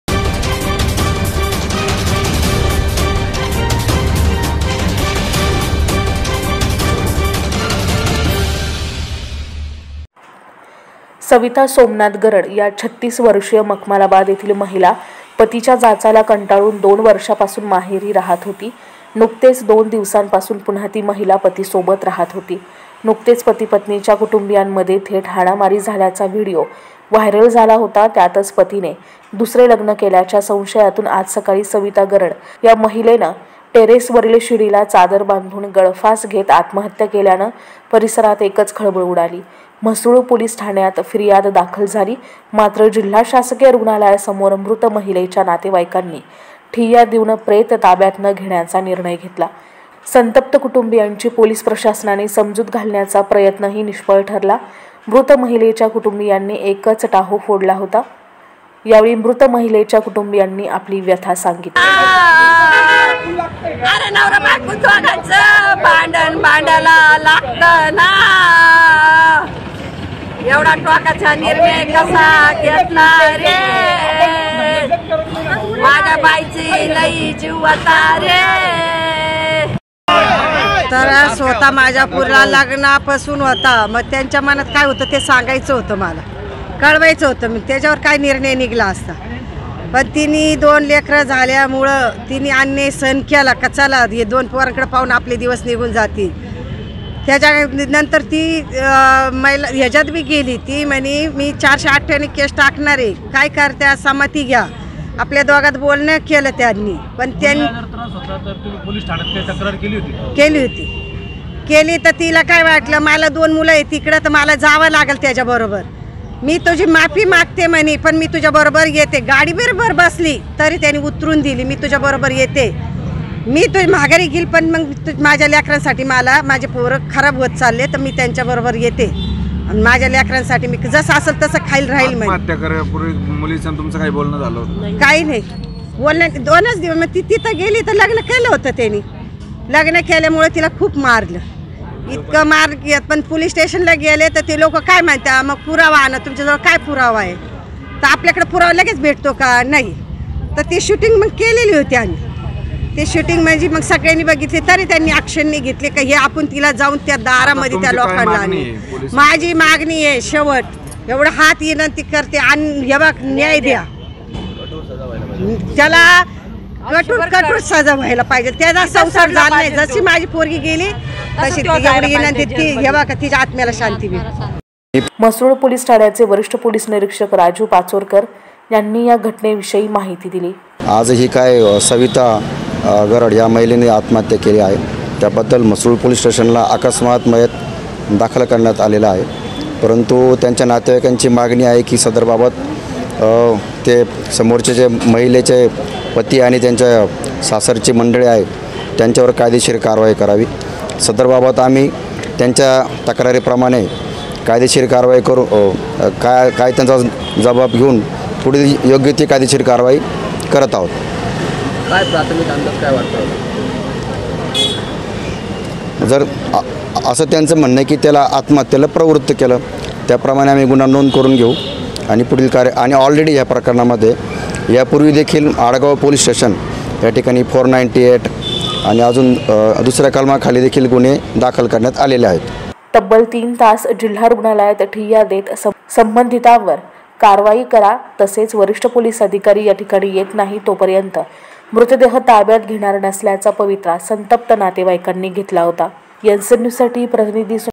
नाशिक । पतिच्या जाचाला कंटाळून पत्नीची आत्महत्या.. नातेवाईकांचा जिल्हा रुग्णालयात ठीय्या..
NCN News